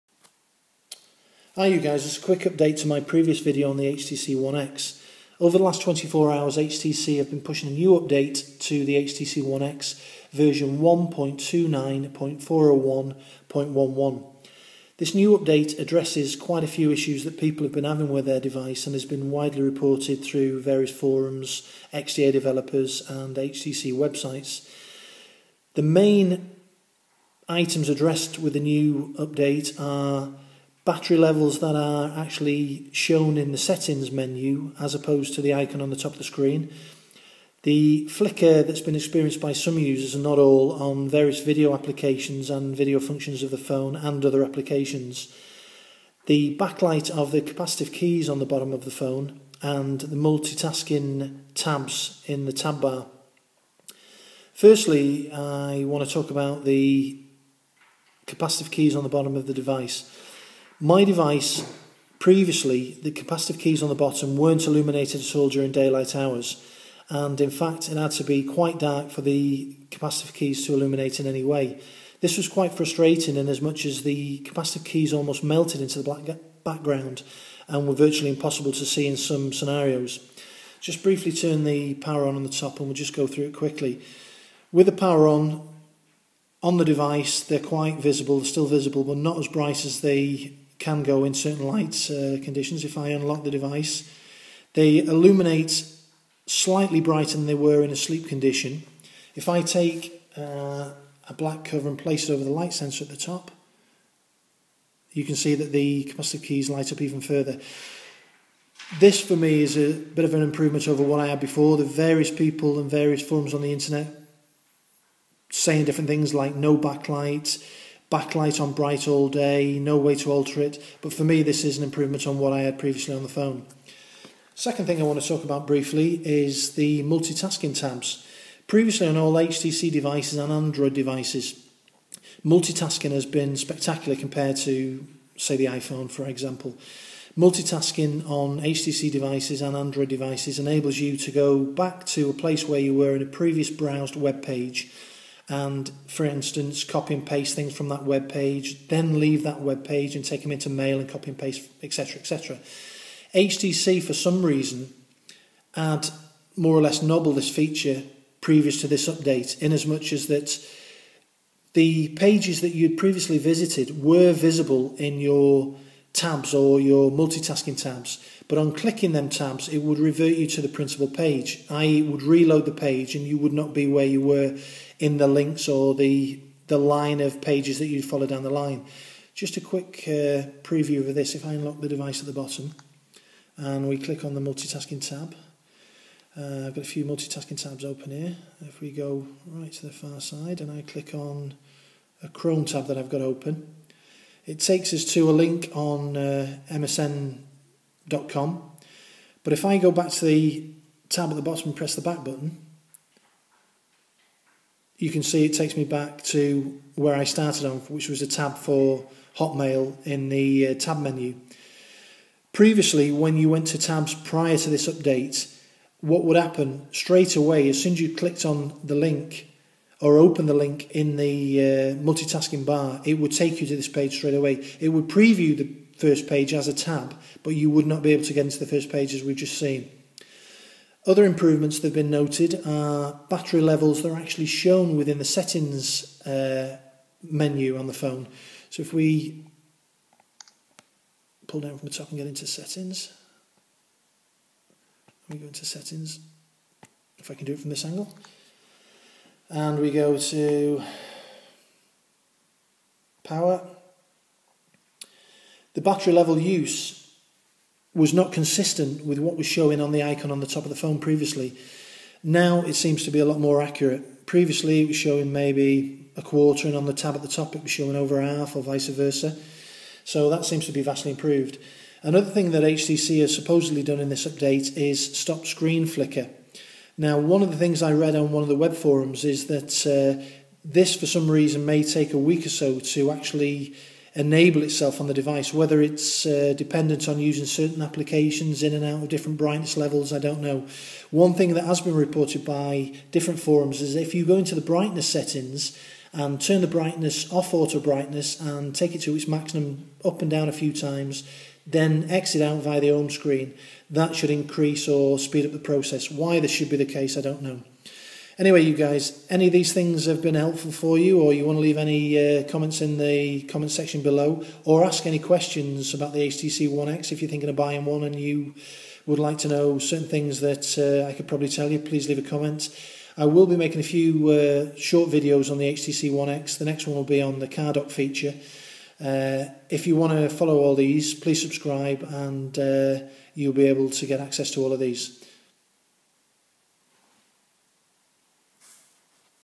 HTC One X update 1.29.401.11
A brief summary of HTC one X software update.
The capacitive keys are actually visible, in the video they are much dimmer than they appear naturally.
Battery level in the settings that match the visible icon on the top bar, both showing equal amounts.
This video is off the cuff, so expect a few mistakes :)